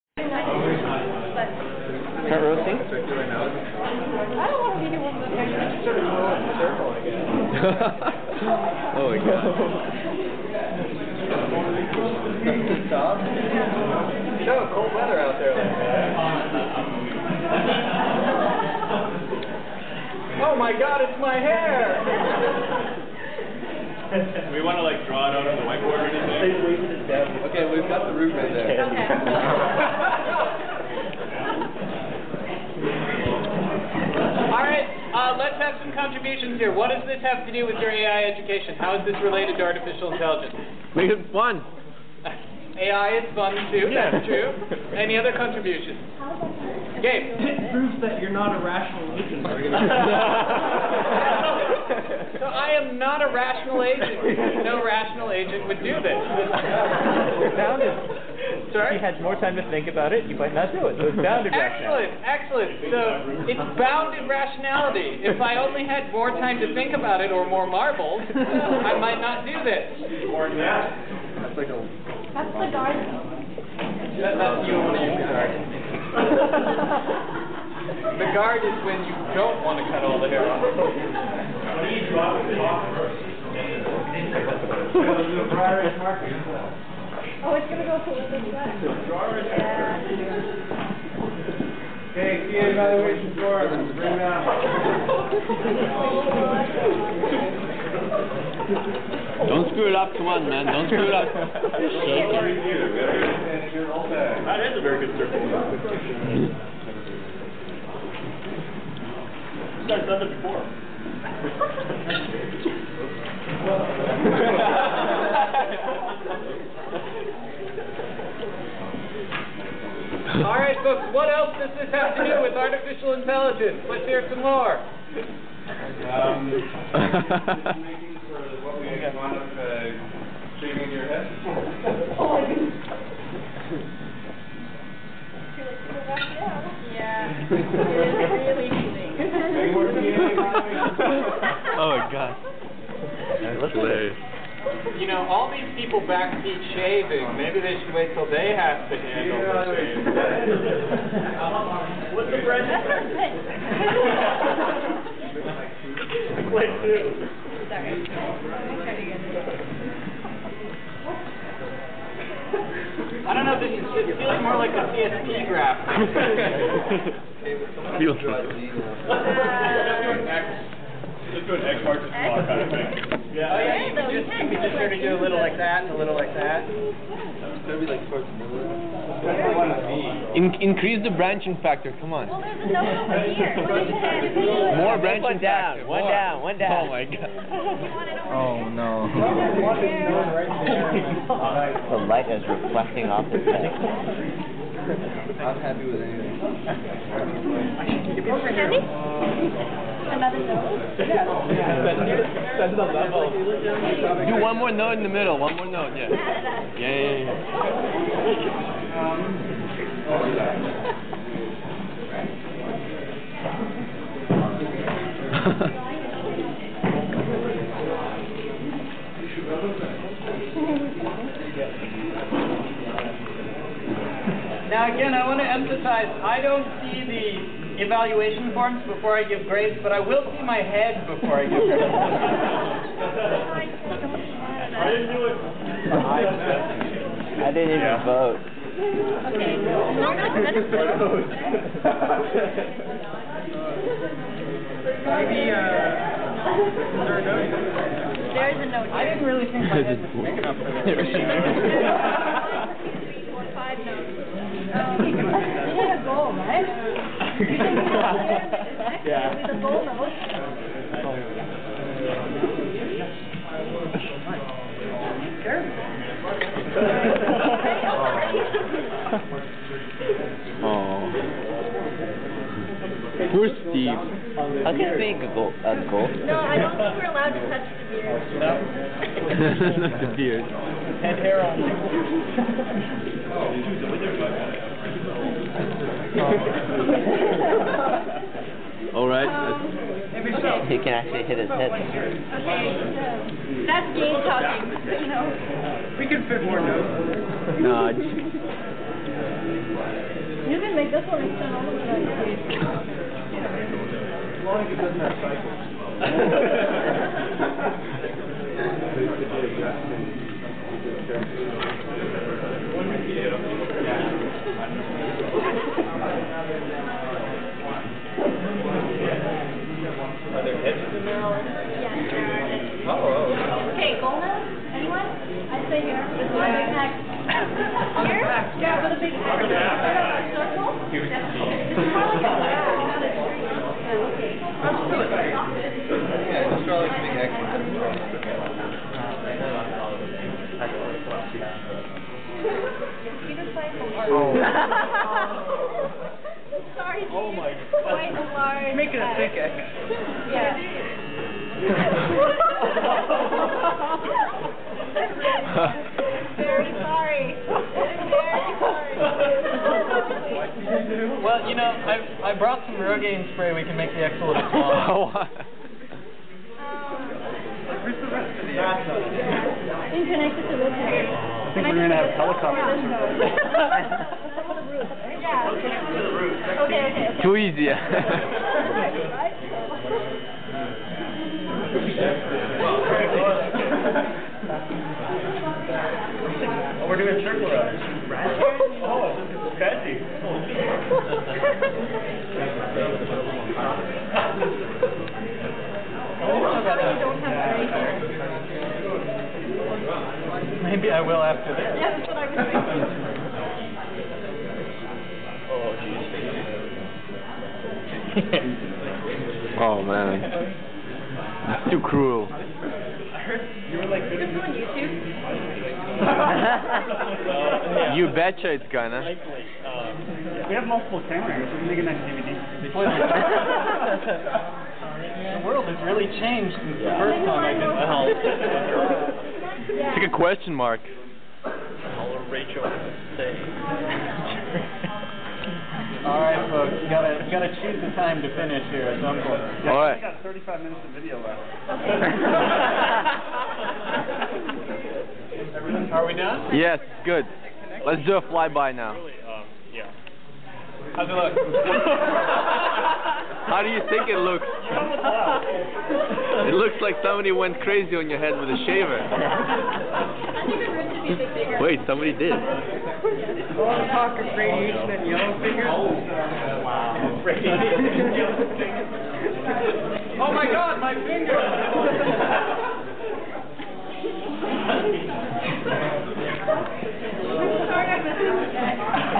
The Great Wolfman Shearing
Steve Wolfman, a University of British Columbia faculty member, lets students shave a shape into his head after the 2006W Computer Science 322 (Intro to Artificial Intelligence) class earned over 128 bonus points.

The shape is supposed to be a search tree with the goal node denoted by an X.

Steve is certainly the most fun prof I ever had.